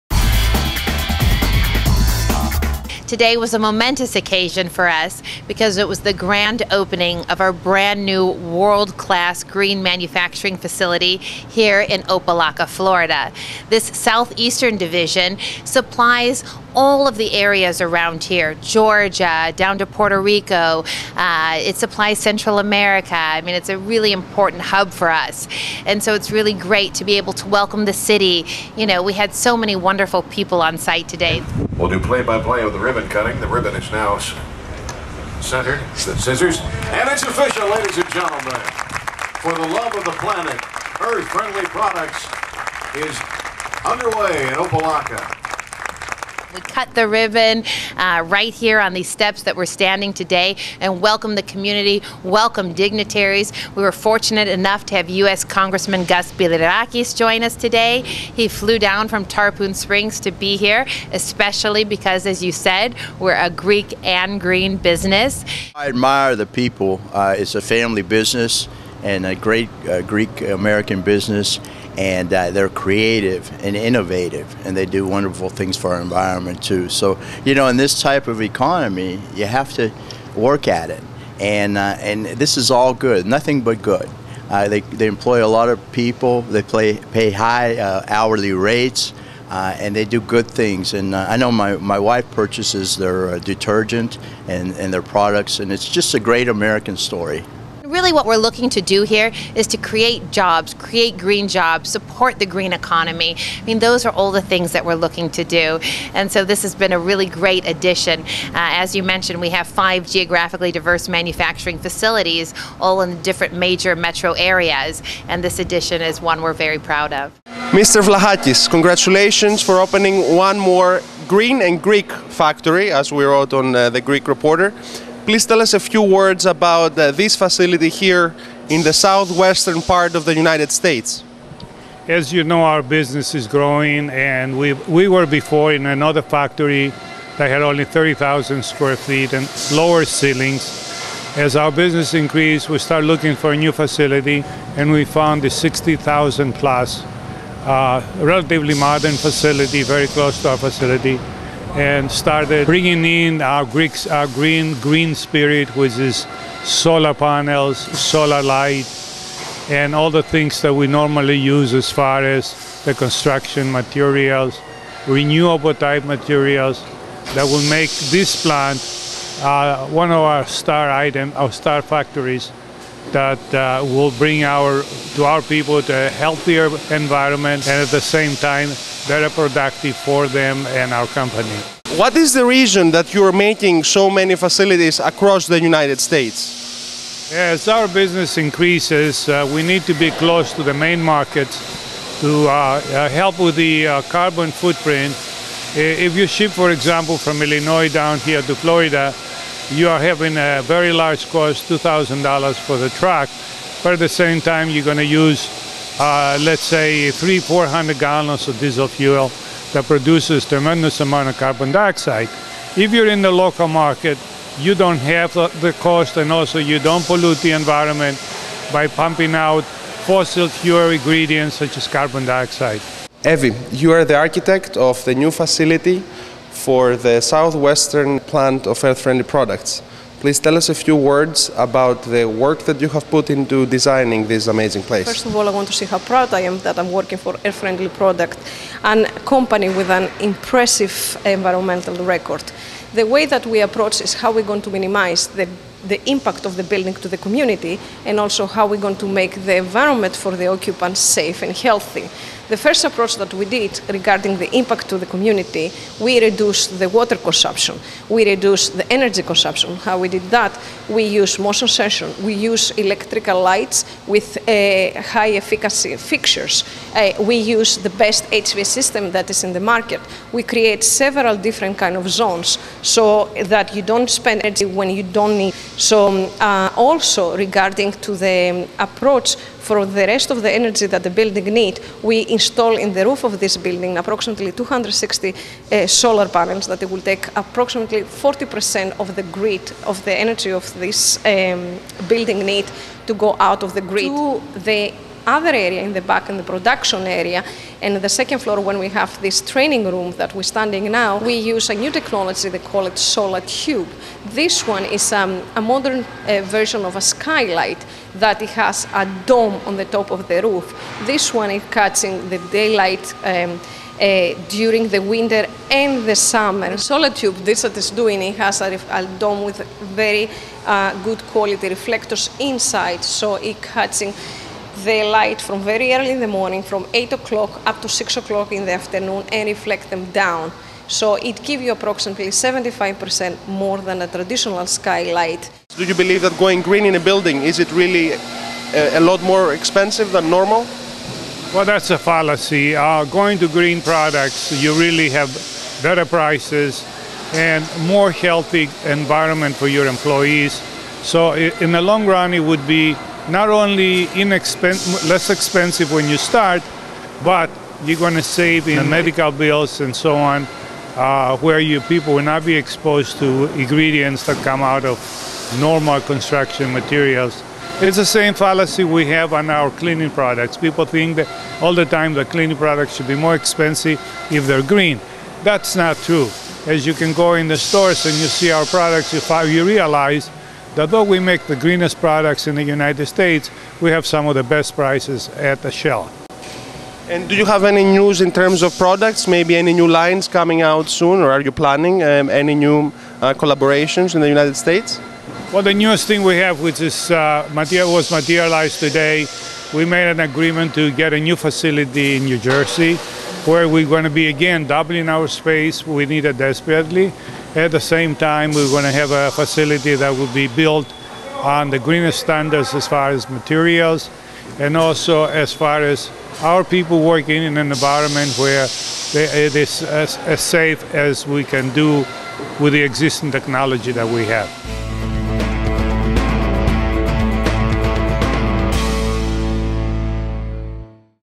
Earth Friendly Products Opens New Green Plant in FL
The maker of ECOS, the world's top-selling green laundry detergent kicked off the gala with a "green carpet" event, a ribbon-cutting, a tour for VIPs and the media, and a special luncheon for honored guests. Watch the interviews with EFP executives and take a sneak pick of the factory.